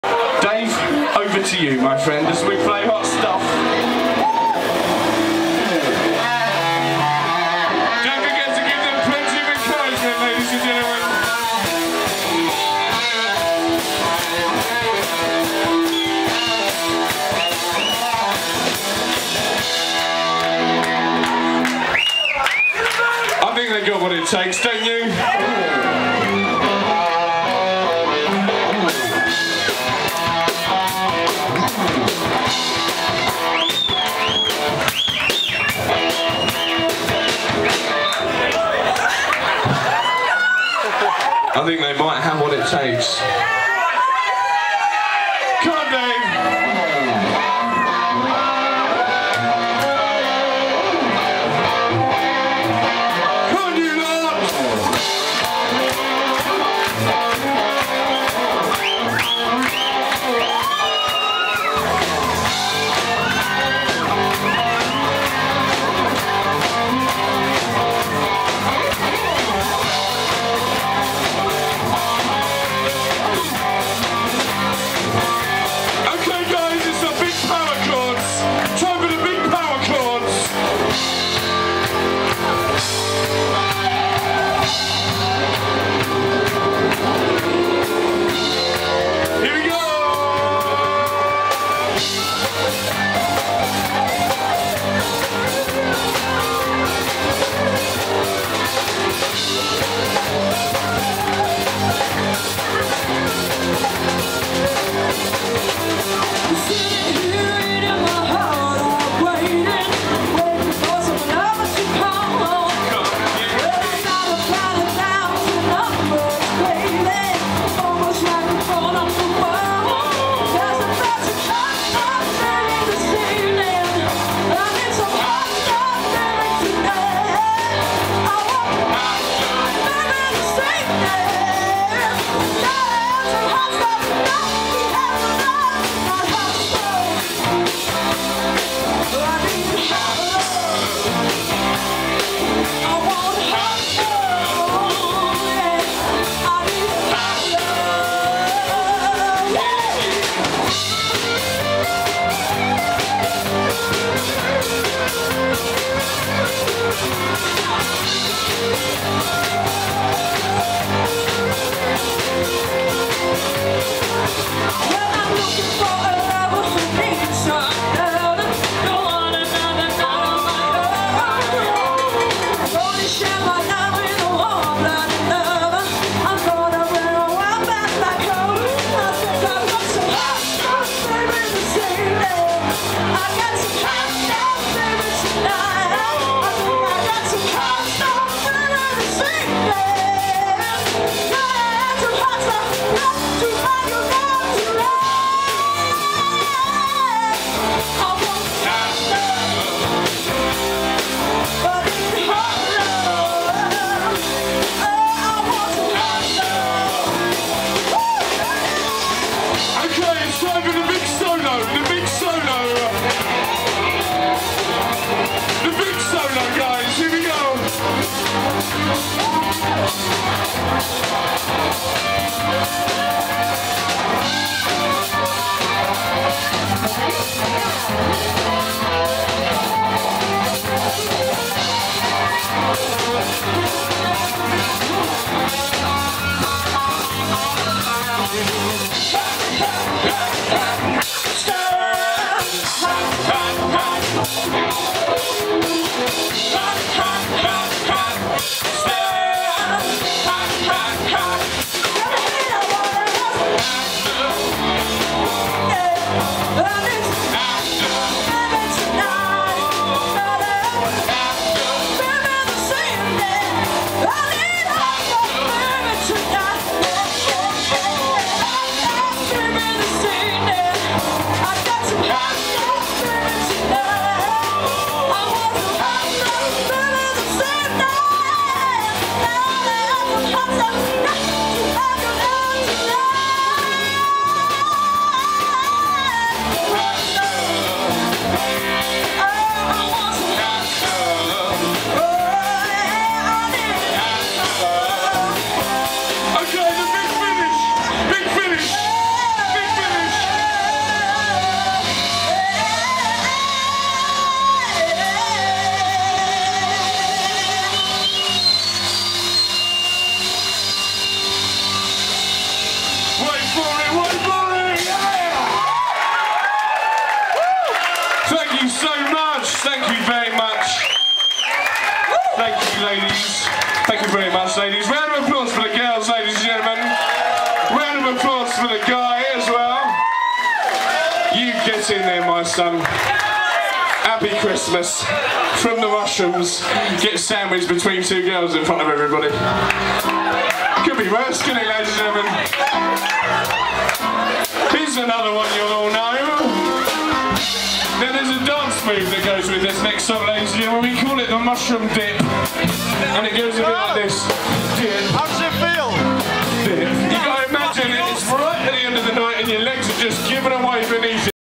Air Guitar With The Mushrooms
Uncle Al and co get all musical with the local band!